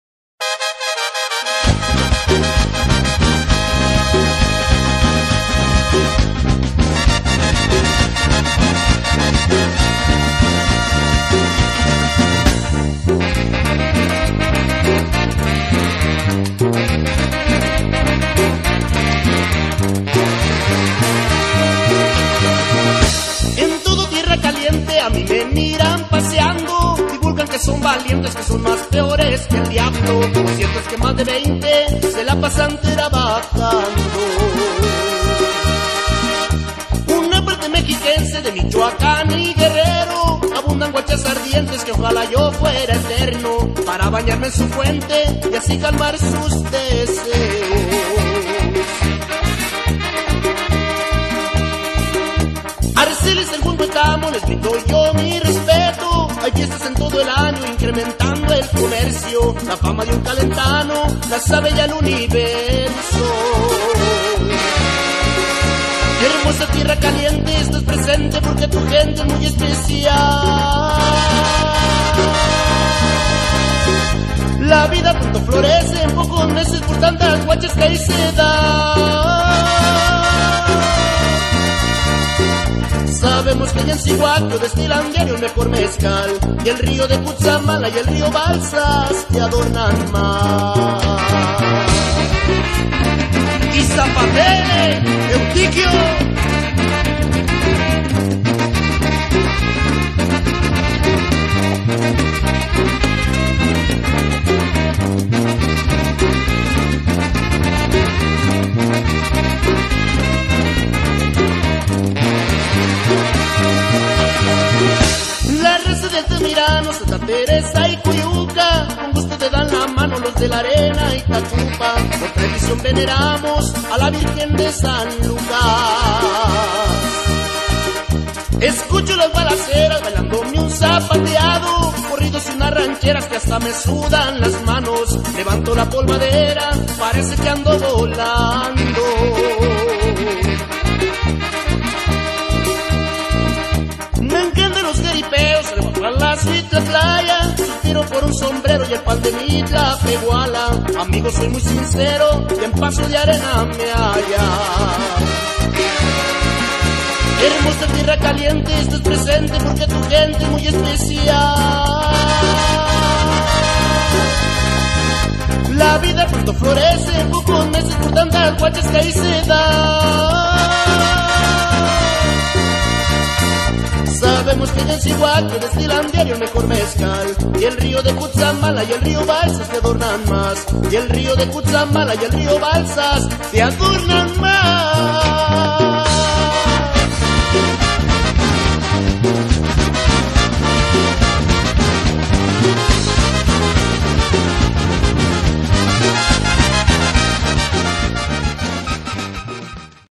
Orgulloso De Ser De Valle Galeana Gro!
Recordando Un Poquito De Mi Pueblo Querido 😢😢 ValleGaleana, Como Se Extraña Andar En Mi Pueblito 😢 Saluds Huaches De Valle!. ..